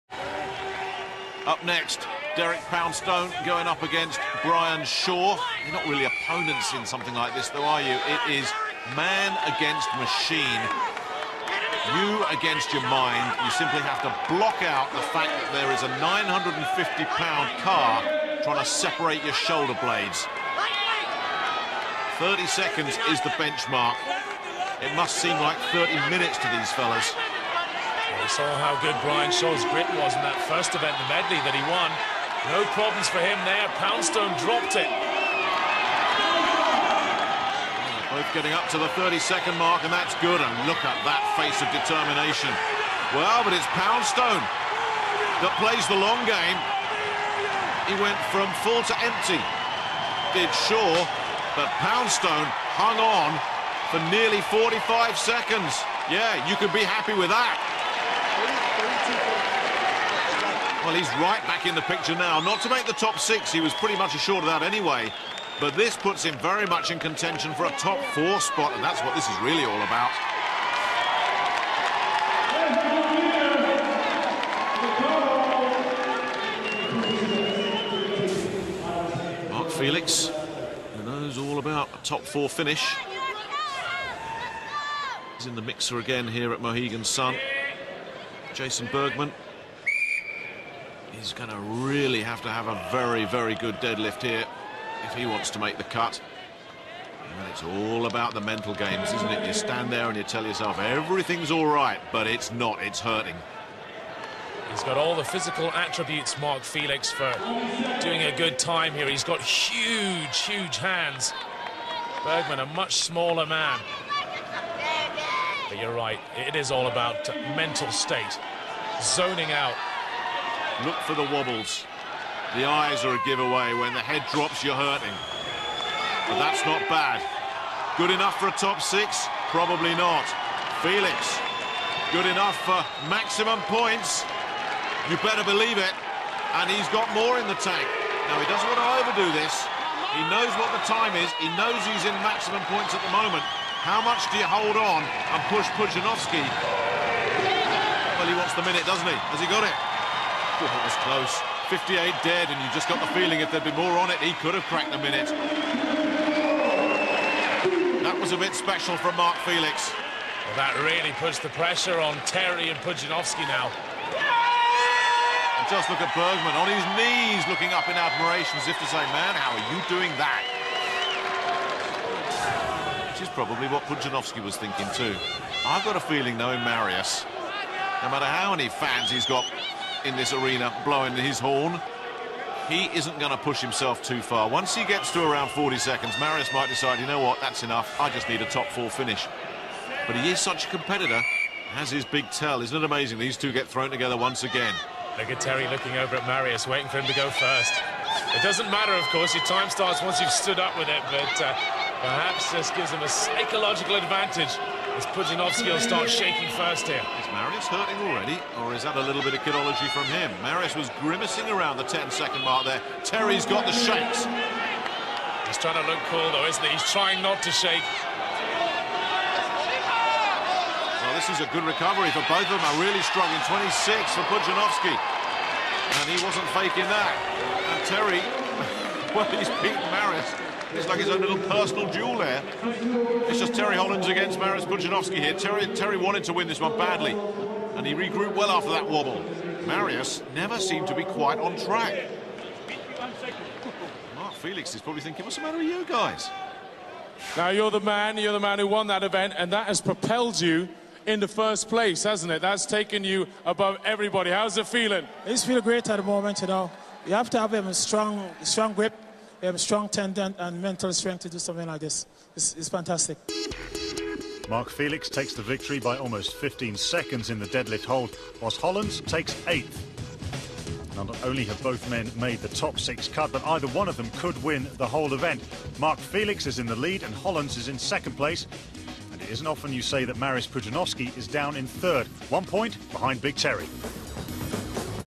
Mark FELIX - Grip KING Smokes Strongman LEGENDS in Car DEADLIFT HOLD
Who has the World’s Strongest GRIP?

Is it 5x World’s Strongest Man Mariusz PUDZIANOWSKI?
Is it 4x World’s Strongest Man Brian SHAW?
Is it the Legends of Strength Derek POUNDSTONE or Terry HOLLANDS?

Watch MARK ‘The Grip King’ FELIX blitz the field in the 430Kg/950lbs Car DEADLIFT Hold from Mohegan Sun 2008.